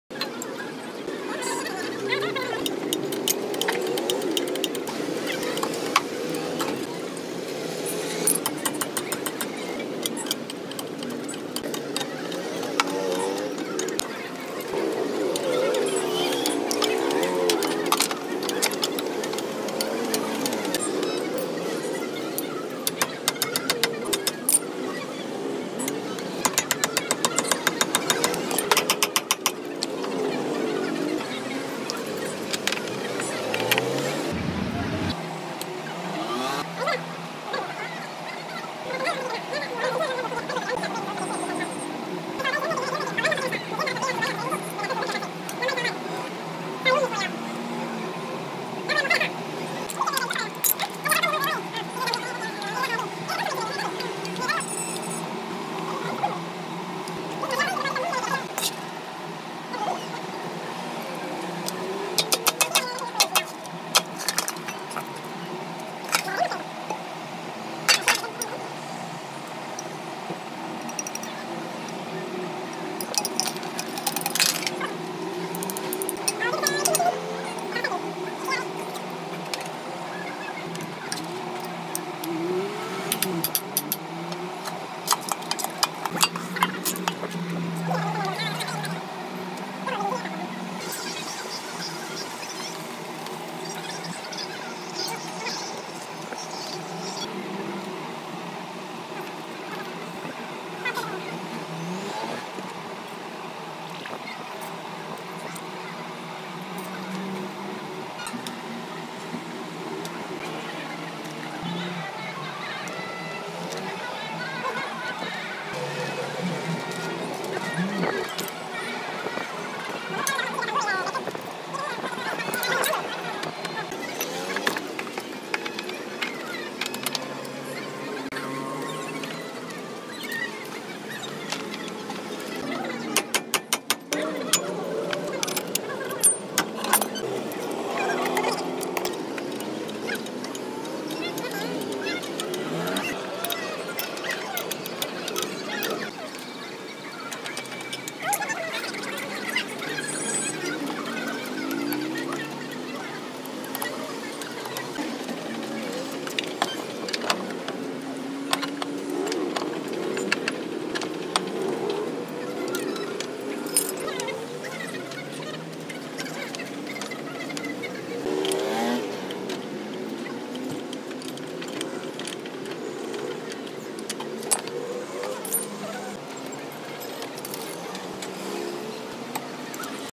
Repairing Car Front Shock Absorber
The shock absorbers in your car play an essential function of keeping your tyres on the road, so you feel stable and secure when on your travels.